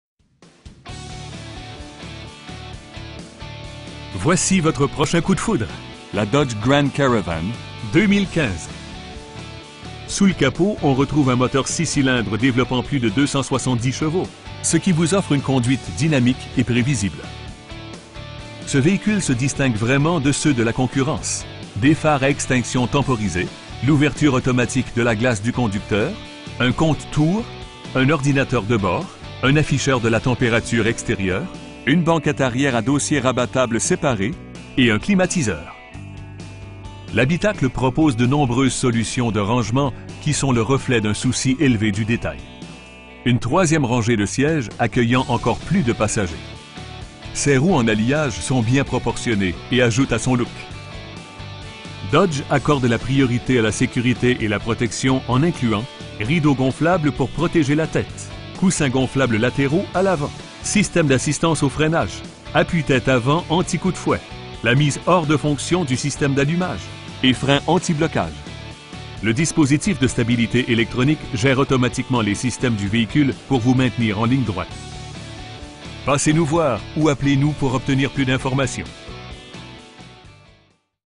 2015 Dodge Grand Caravan SXT Premium Plus
Voici votre prochain coup de foudre: la Dodge Grand Caravan 2015. Sous le capot on retrouve un moteur 6 cylindres d��veloppant plus de 270 chevaux, ce qui vous offre une conduite dynamique et pr��visible. Ce v��hicule se distingue vraiment de ceux de la concurrence: des phares �� extinction temporis��e, l'ouverture automatique de la glace du conducteur, un compte-tours, un ordinateur de bord, un afficheur de la temp��rature ext��rieure, une banquette arri��re �� dossiers rabattables s��par��s, et un climatiseur. L'habitacle propose de nombreuses solutions de rangement qui sont le reflet d'un souci ��lev�� du d��tail. Une 3e rang��e de si��ges accueillant encore plus de passagers. Ses roues en alliage sont bien proportionn��es et ajoutent �� son look. Dodge accorde la priorit�� �� la s��curit�� et la protection en incluant: rideaux gonflables pour prot��ger la t��te, coussins gonflables lat��raux �� l'avant, syst��me antipatinage, syst��me d'assistance au freinage, appuis-t��te avant anti-coup de fouet, la mise hors de fonction du syst��me d'allumage, et freins antiblocage. Le dispositif de stabilit�� ��lectronique g��re automatiquement les syst��mes du v��hicule pour vous maintenir en ligne droite. Ce qui nous rend fiers, c'est vous offrir uniquement des v��hicules de grande qualit��. Passez nous voir ou appelez-nous pour obtenir plus d'information.